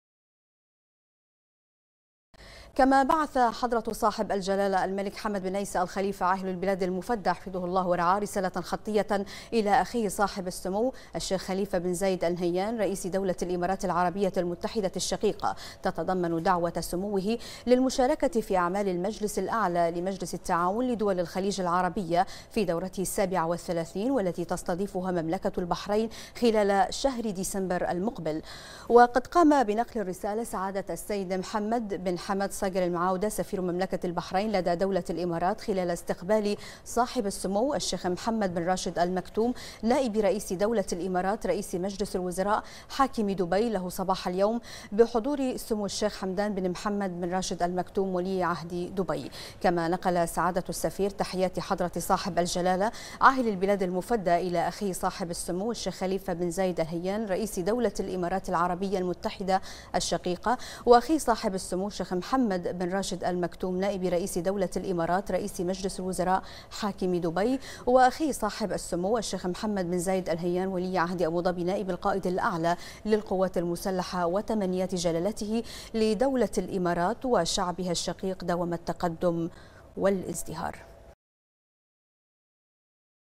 البحرين: عاهل البلاد المفدى يبعث رسالة خطية إلى سمو رئيس دولة الإمارات العربية المتحدة
أبوظبي في 28 نوفمبر / بنا / بعث حضرة صاحب الجلالة الملك حمد بن عيسى آل خليفة عاهل البلاد المفدى حفظه الله ورعاه رسالة خطية إلى أخيه صاحب السمو الشيخ خليفة بن زايد آل نهيان رئيس دولة الامارات العربية المتحدة الشقيقة، تتضمن دعوة سموه للمشاركة في أعمال المجلس الأعلى لمجلس التعاون لدول الخليج العربية في دورته الـ 37 والتي تستضيفها مملكة البحرين خلال شهر ديسمبر المقبل.